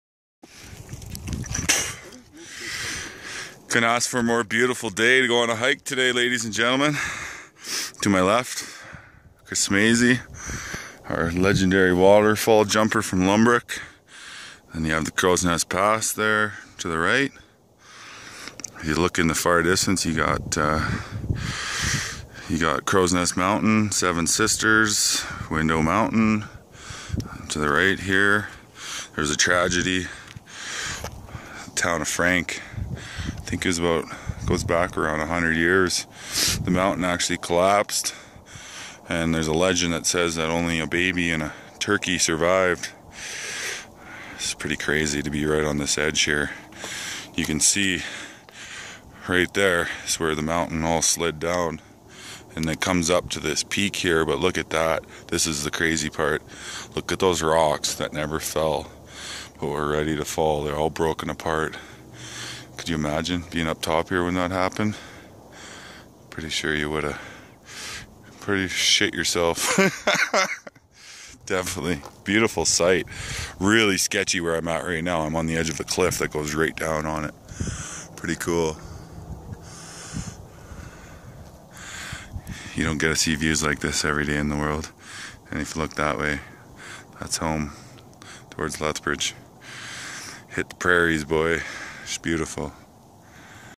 Most beautiful mountain top view Frank slide!
2 men sit a peak of famous mountain that came down on historical coal mine town! Beautiful view!